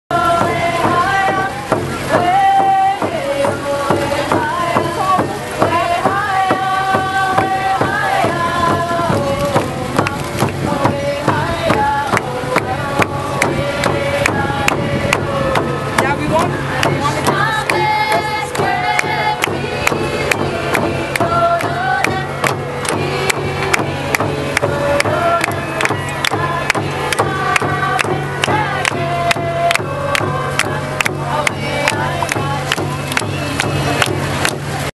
Drumming @ Solidarity March 5 July -- Women's Worlds 2011
A clip featuring Aboriginal drummers who played on Parliament Hill as part of the Solidarity March in support of missing and murdered Aboriginal women on 5 July as part of Women's Worlds 2011.